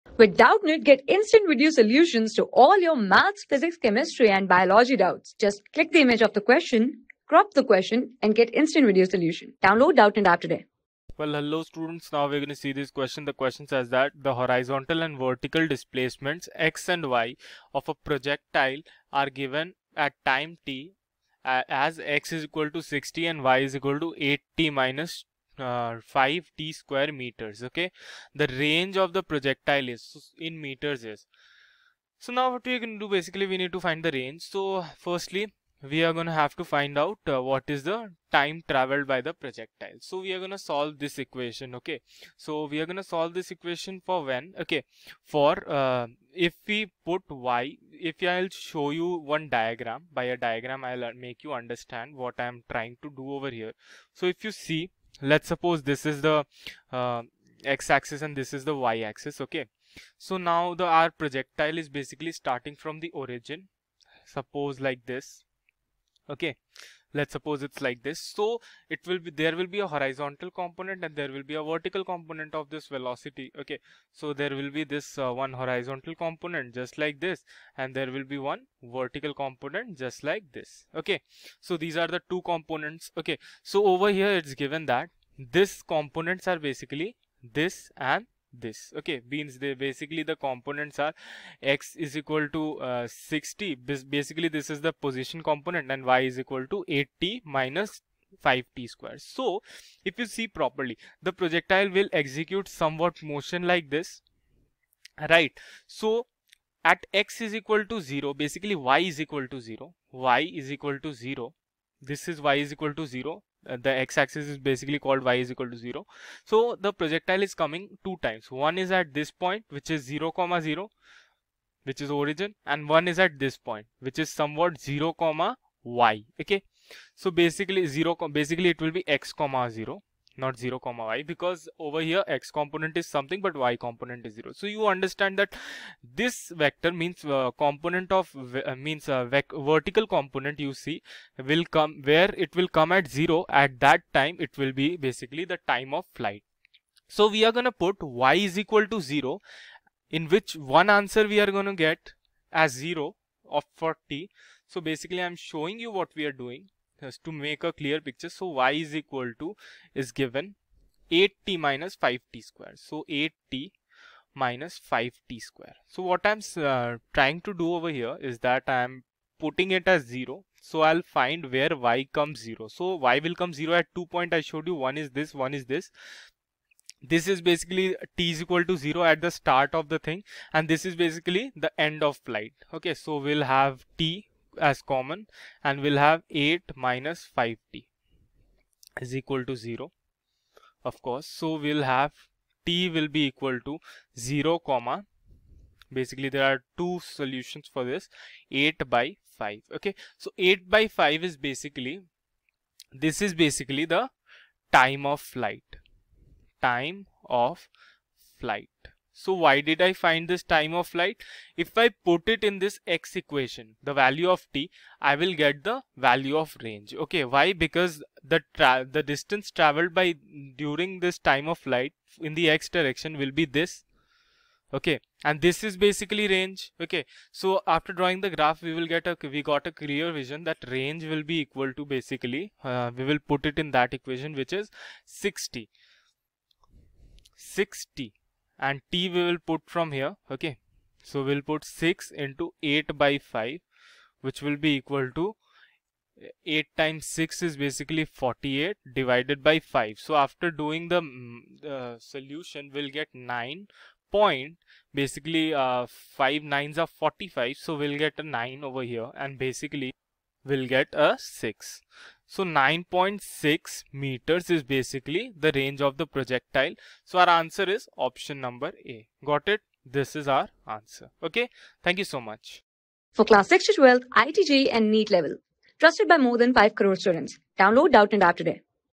The horizontal and vertical displacements x and y of a projectile at a given time t are given by...
The horizontal and vertical displacements x and y of a projectile at a given time t are given by x= 6t and y= 8t -5t^2 meter.The range of the projectile in metre is:
Class: 12
Subject: PHYSICS
Chapter: Motion in Two Dimensions
Board:IIT JEE

👉 Have Any Query? Ask Us.